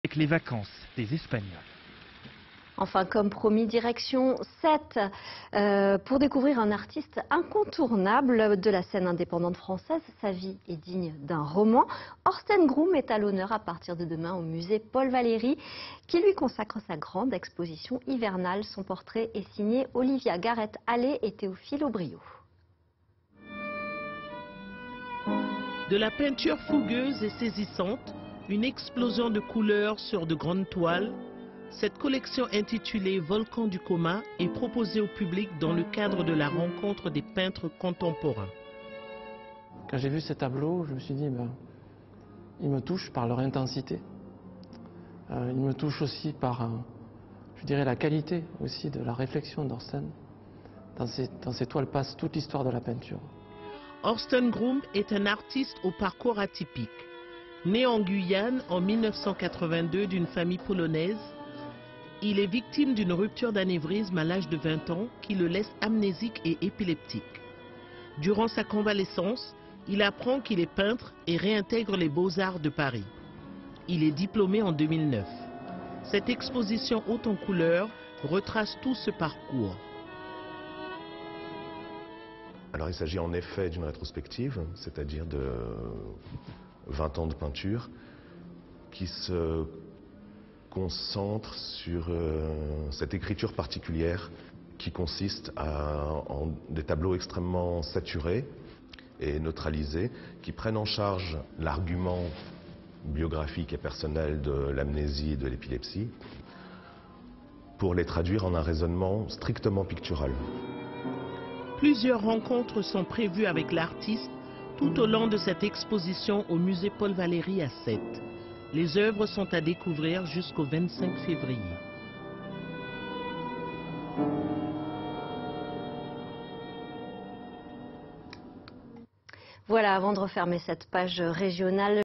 Orsten Groom - Rétrospective VOLCAN DU COMA au Musée Paul Valéry, Sète
Le musée Paul Valéry de Sète présente la première rétrospective muséale du peintre Orsten Groom - VOLCAN DU COMA - du 1er décembre 2023 au 25 février 2024